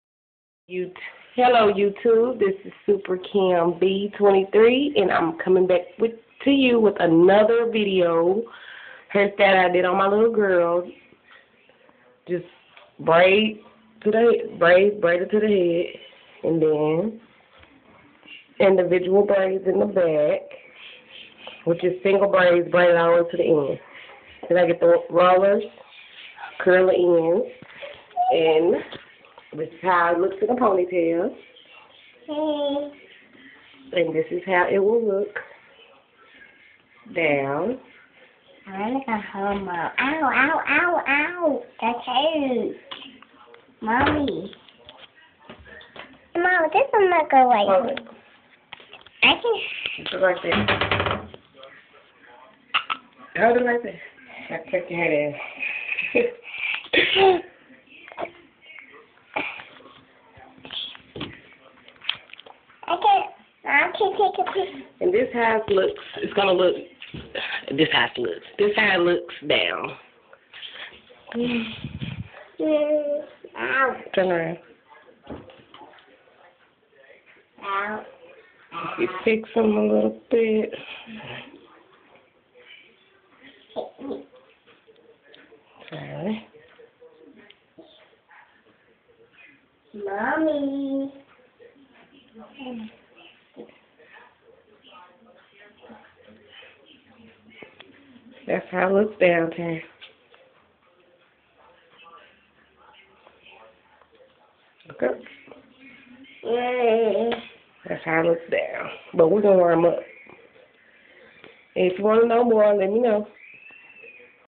Braids on my little girl hair
This is one of the hair styles I put on my daughters hair,this last for like 3 to 4 weeks,maybe longer depends on how u take care of it
IG : IDOHAIRIDONAILS ( PLEASE FOLLOW )